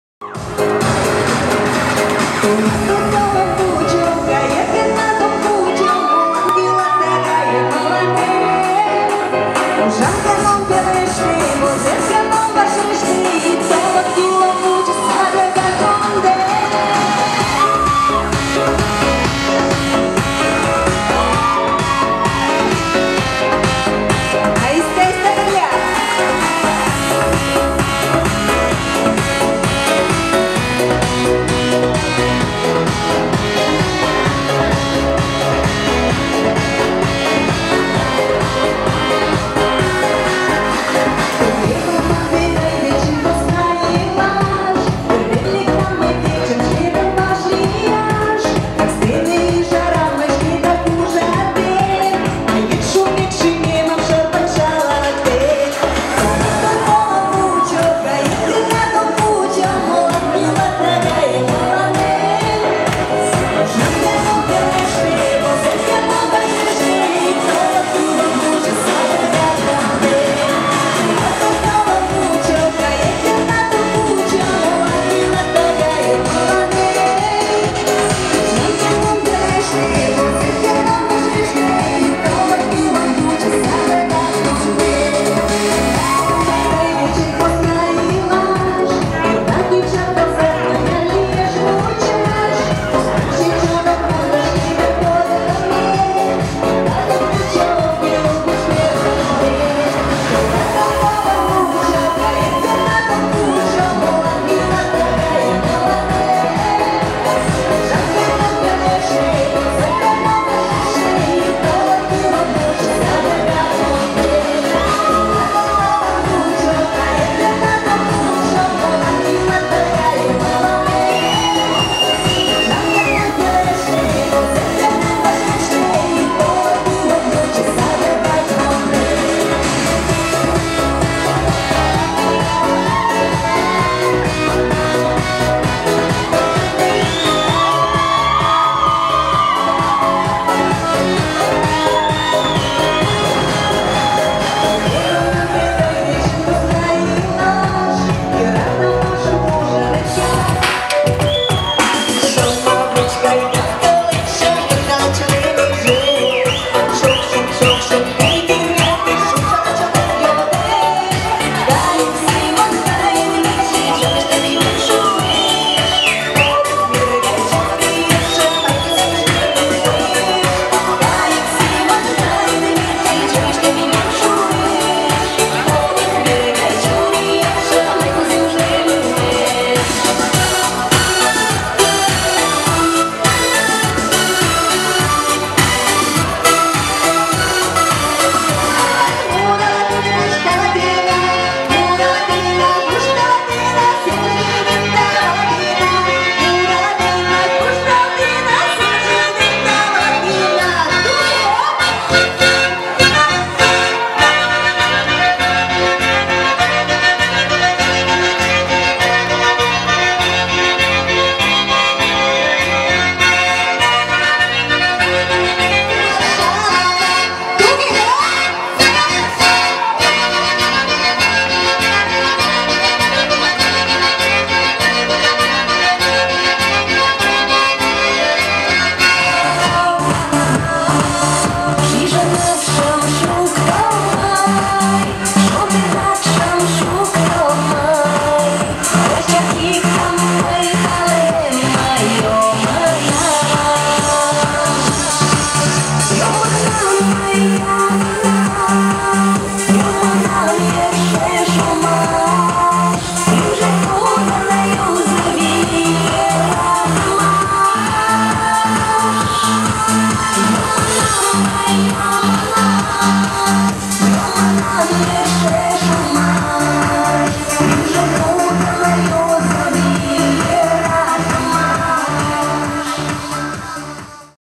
Поет Татьяна Соловьева на Мари Пати 19 12 2013 г
Екатеринбург, кафе "Golden Fish"